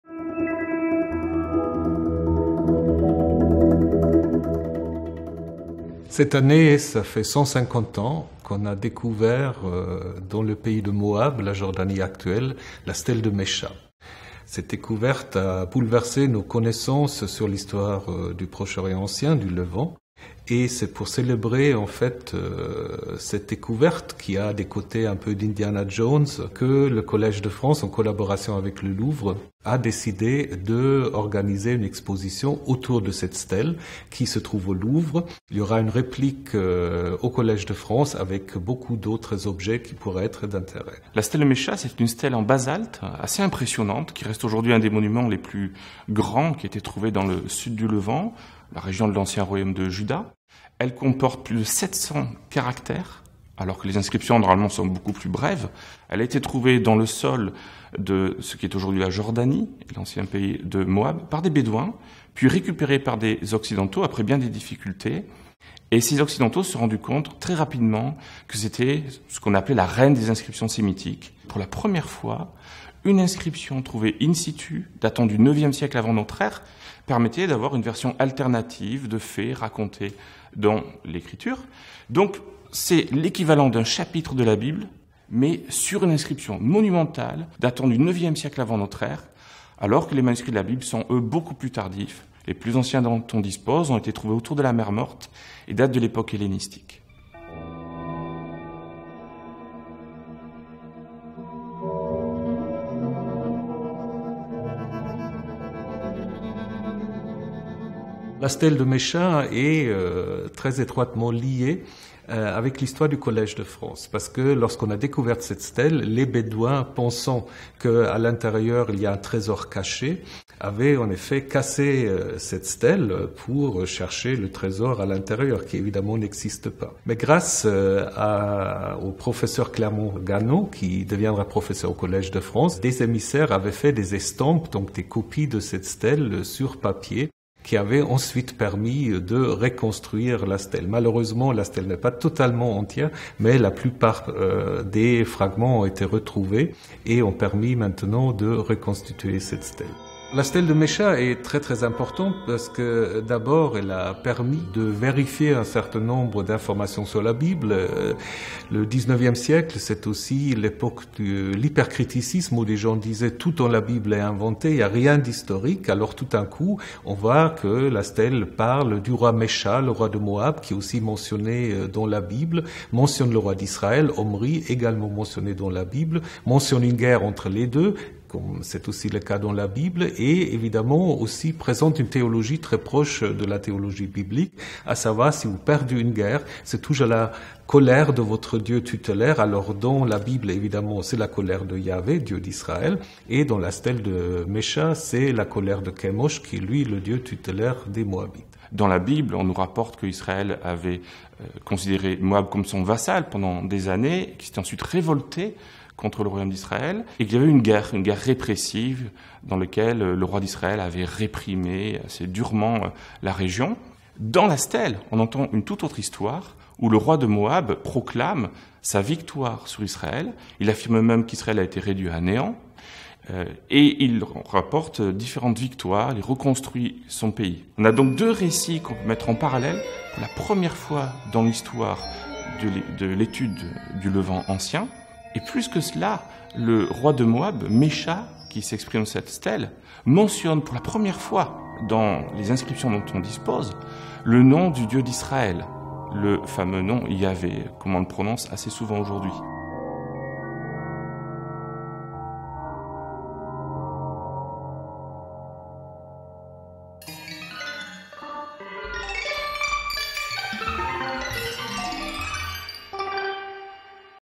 Mésha et la Bible : quand une pierre raconte l'histoire
Il y a 150 ans, en 1868, un missionnaire alsacien du nom de Klein découvrit dans le pays de l’ancien Royaume de Moab, la Jordanie actuelle, une stèle en basalte noir d’une hauteur d’environ 120 centimètres. Cette découverte fit beaucoup de bruit et les Bédouins, pris dans des négociations tendues avec les Européens, et imaginant peut-être aussi qu’un trésor se trouvait à l’intérieur, allèrent jusqu’à détruire la stèle. Évidemment, il n’y avait rien ; mais la magnifique stèle avait été brisée. C’est grâce à la perspicacité de Charles Clermont-Ganneau, qui deviendra plus tard professeur du Collège de France, que cette stèle a pu être reconstruite. Il avait demandé à des émissaires d’en réaliser un estampage qui a ensuite permis, comme dans un jeu de puzzle, de recoller presque tous les morceaux.

Pourquoi cette stèle est-elle si importante ? Elle date du IXe siècle avant l’ère chrétienne et contient la première mention des quatre lettres du dieu d’Israël (souvent prononcé Yahvé) en dehors de la Bible. Elle est écrite dans une écriture alphabétique et mentionne des événements qui sont aussi relatés, quoique de manière très différente, dans le texte biblique.

Avec la participation exceptionnelle du musée du Louvre, grâce au concours du musée Bible et Terre Sainte (Institut Catholique de Paris), du musée national de Beyrouth, de l’Académie des inscriptions et belles-lettres, du Service historique de la Défense et de la Société française d’histoire du protestantisme, le projet scientifique et culturel de l’exposition consiste à mettre en valeur l’importance de la stèle de Mésha sur trois plans.

Tout d’abord, il s’agit de situer cette découverte dans le contexte des débuts de l’archéologie du Levant pour laquelle elle fut un événement fondateur. L’exposition retrace la découverte, la destruction et la reconstitution de la stèle grâce à l’implication de Charles Clermont-Ganneau. On peut notamment y découvrir une réplique de la fameuse stèle exposée au musée du Louvre et surtout l’estampage original, accessible au grand public pour la première fois depuis le milieu du siècle dernier. L’exposition montre ensuite la place centrale de la stèle pour l’étude des écritures du Levant ancien et l’histoire de l’alphabet. On y découvre différents exemples d’écritures alphabétiques anciennes. Un atelier permet aussi de se confronter plus directement à elles.

Enfin, l’exposition explique l’importance de ce monument pour les études bibliques. En effet, la stèle présente une vision de l’intervention du dieu tutélaire de Moab qui peut être comparée à la fonction du dieu Yahvé pour Israël. Des objets rares illustrant le contexte religieux du Levant ancien donnent une profondeur historique au parcours de l’exposition, qui permet de mieux mener cette réflexion.

En résumé, cette exposition permet de découvrir des objets et des documents aux origines de l’aventure de l’archéologie. La stèle de Mésha n’a pas fini de parler ! Elle demeure une source unique pour l’intelligence du Levant ancien.

Avant-propos du catalogue de l'exposition.
Thomas Römer, Professeur titulaire de la chaire Milieux bibliques